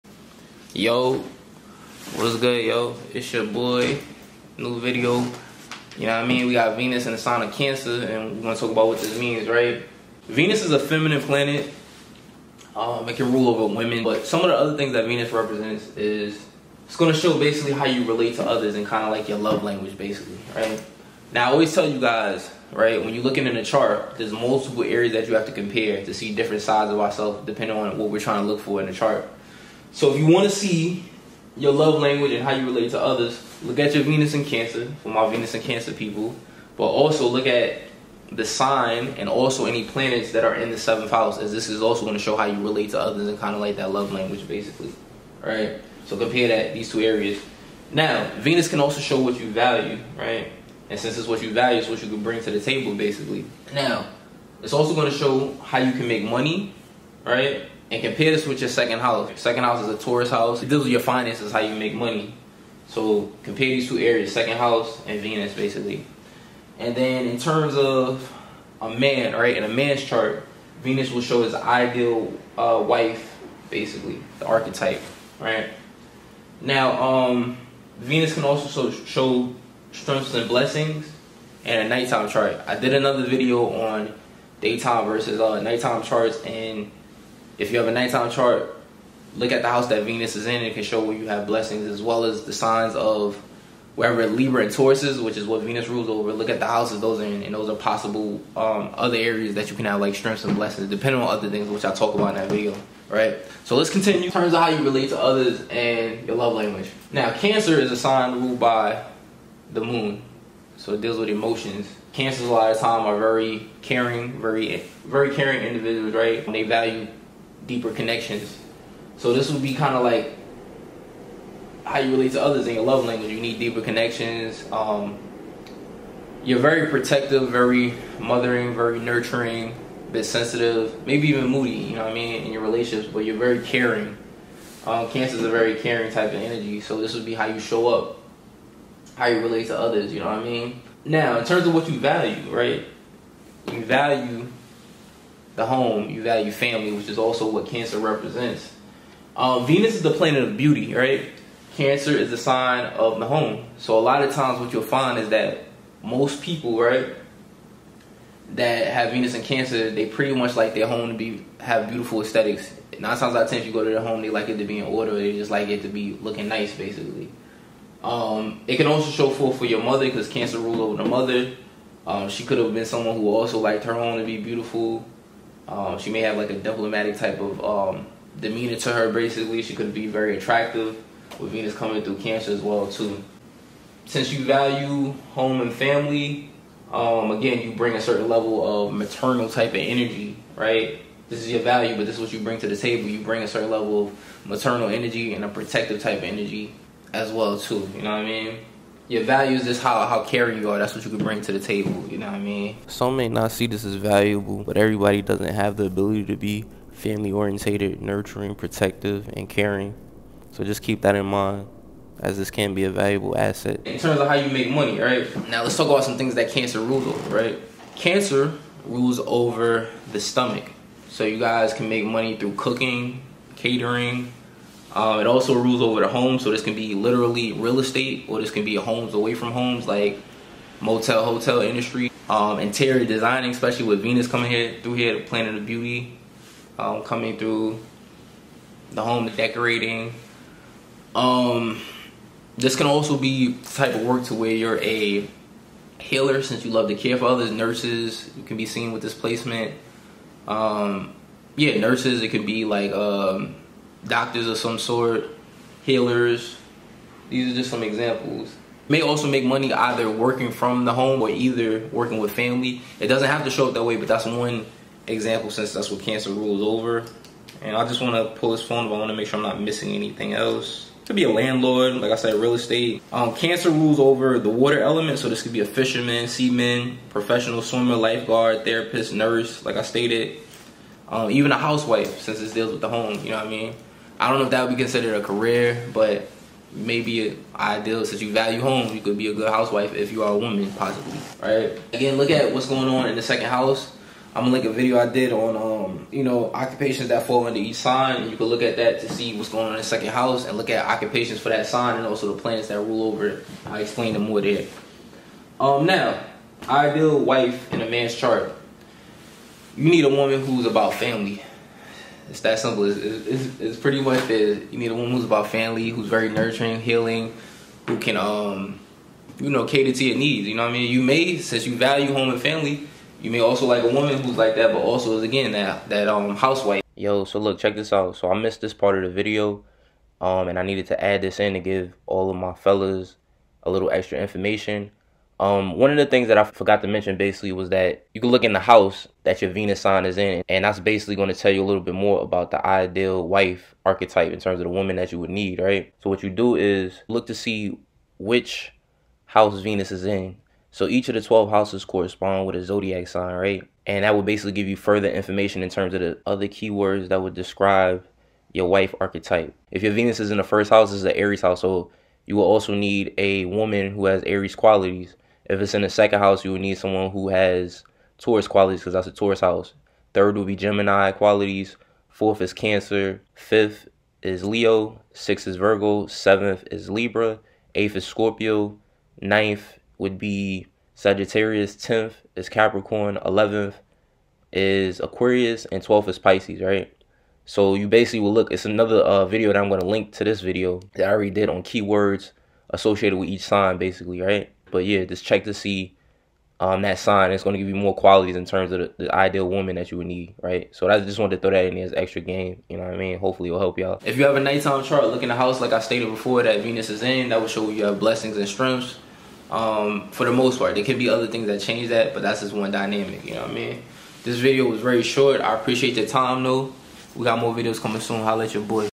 ♋️ NATAL VENUS IN CANCER / NATAL VENUS IN THE 4TH HOUSE (SAME ENERGY)
A Brief Description Of Venus In Cancer.

TIMESTAMPS:
00:00 - Intro
00:10 - What Venus Shows In A Birth Chart
01:44 - How You Relate 2 Others / Love Language (Compare With The 7th House)
03:04 - What You Value/ The Value You Bring To The Table
03:34 - How You Make Money (Compare With Your 2nd House)
05:06 - Ideal Wife Archetype (In A Man’s Chart)
06:44 - Which House You Have Strengths/ Blessings (Night Chart Only)